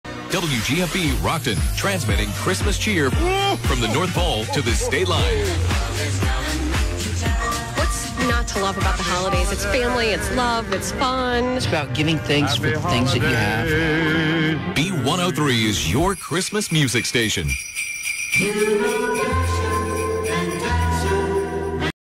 WGFB - B103 - Station ID (6PM) December 23, 2021
The 6pm Station ID for WGFB 103.1 Rockton-Rockford, Illinois. Copyright Mid-West Family Broadcasting.